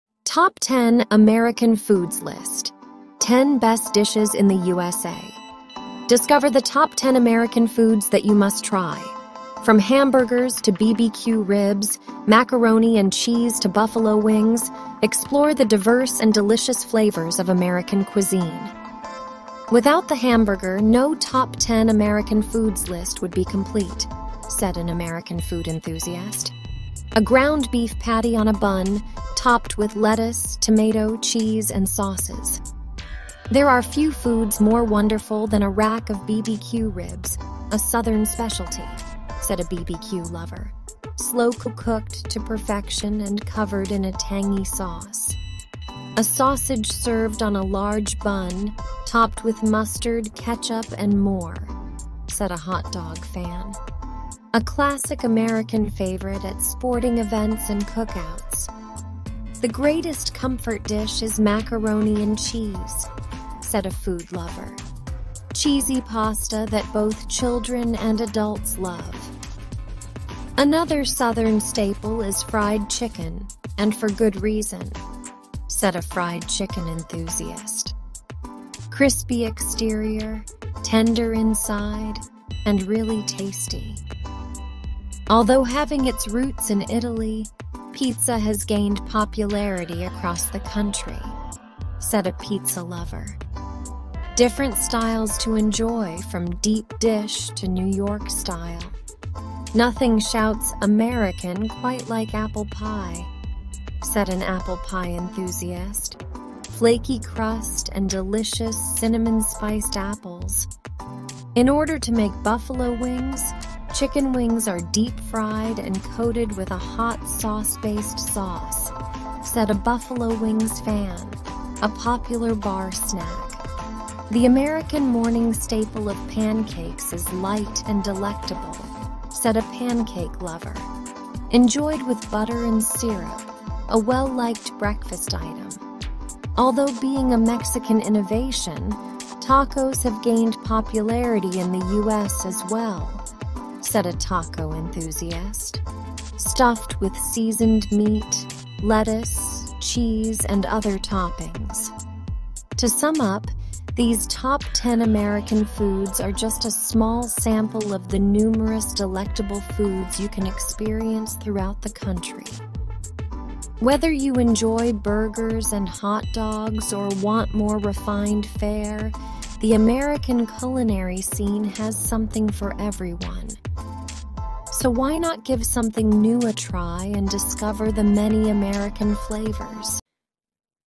Incredible Top 10 Most Popular American/USA Foods ‼️
"Embark on a mouthwatering journey through American cuisine with our guide to the top 10 most popular foods in the USA! From classic burgers to indulgent apple pie, this video is your ultimate culinary adventure through American gastronomy.

Ranking The Best Foods In America
American/USA Top 10 Most Popular Traditional Foods/Dishes You Need to Try Must
The first thing that comes to mind when you think of “American cuisine” are classics like burgers, fried chicken, hot dogs, and pancakes. Like many things American, the cuisine also has influences from elsewhere – German, British, Italian, Dutch, French, and the Caribbean are only some of the cultures that have contributed to the food that is now considered typically American. Here’s our roundup of popular American foods that you will find across the country.
Recently uploaded
New to you
History
Trending